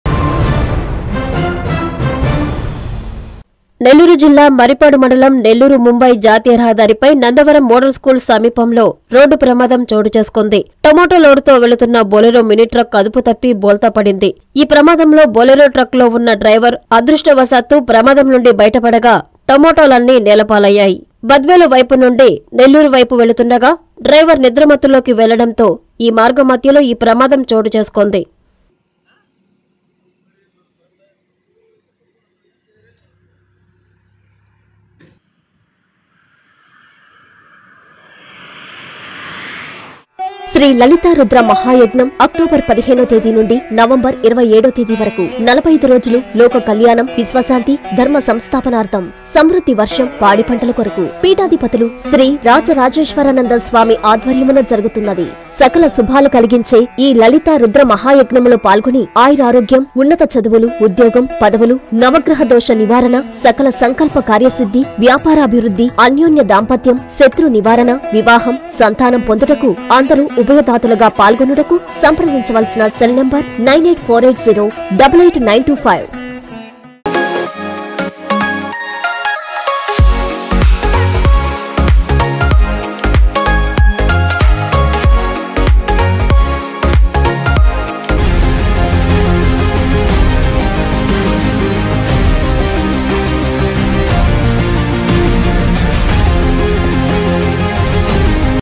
ట‌మోటో లోడ్‌తో వెళుతున్న మినీ ట్ర‌క్ బోల్తా
నేల‌పాలైన ట‌మోటోలు
డ్రైవ‌ర్‌కి త‌ప్పిన ప్ర‌మాదం
మ‌ర్రిపాడులో ఘ‌ట‌న